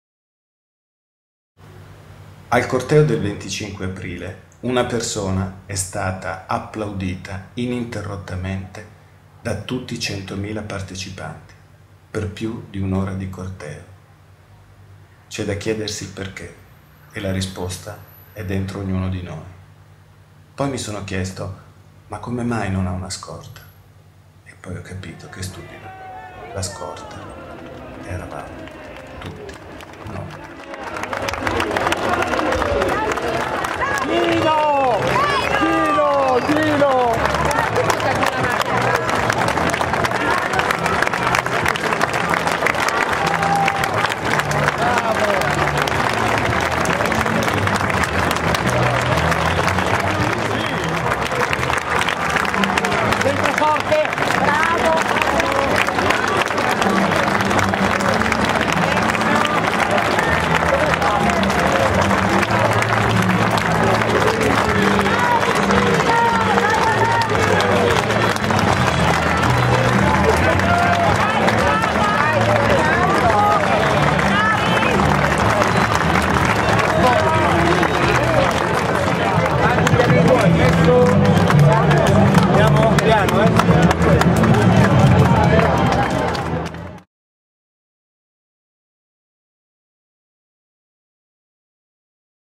25 aprile 2010 milano corteo: Emergency e Gino Strada
milano 25 aprile corteo della liberazione: un uomo viene applaudito ininterrottamente  per un ora da 100.000 persone, non ha scorta , non ne ha bisogno!